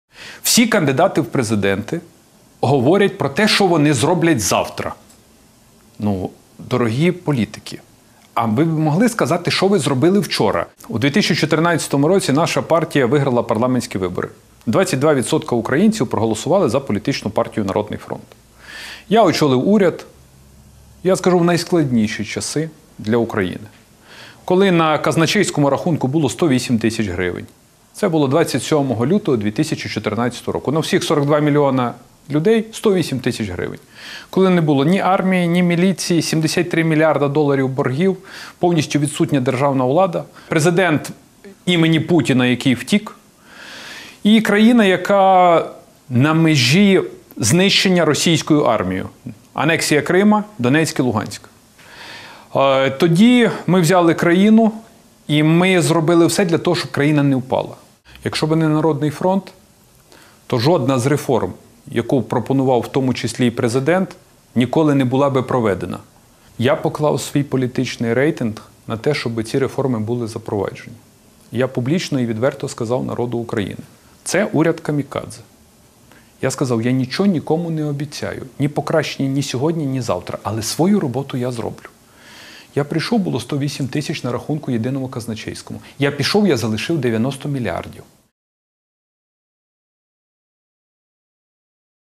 Арсеній Яценюк: Дорогі політики, а ви б могли сказати, що ви зробили вчора?
Приєднуйся!